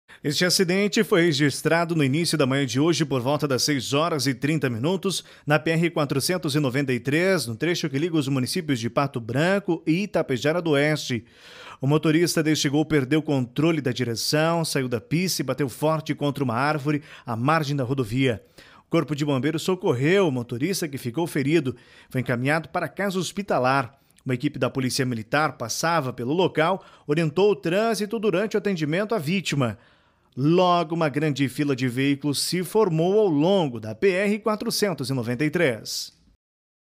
Carro sai da pista e bate forte contra uma árvore na PR-493
Segurança: O acidente foi registrado na manhã de hoje (17) na PR-493.